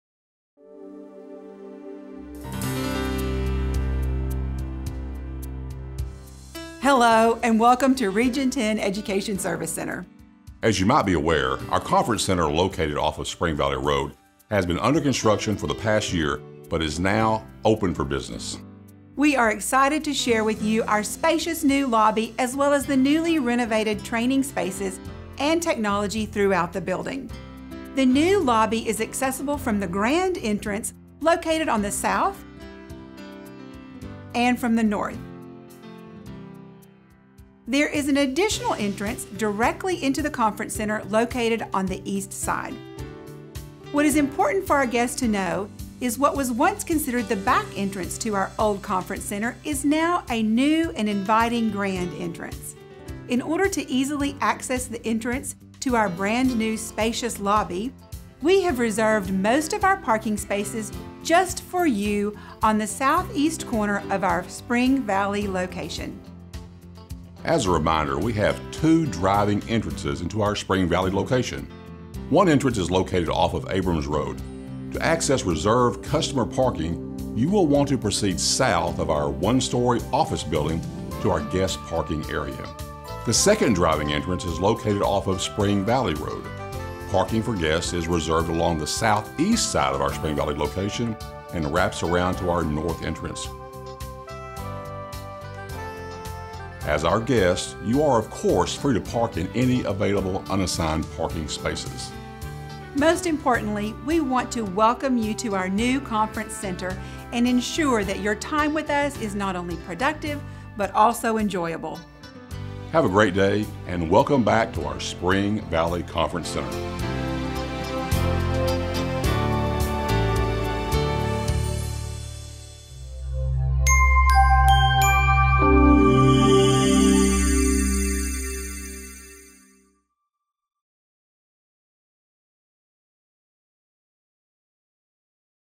Region 10 ESC | Welcome Back to Spring Valley Conference Center
Check out this short video to see all of the amazing updates made to our Spring Valley Conference Center.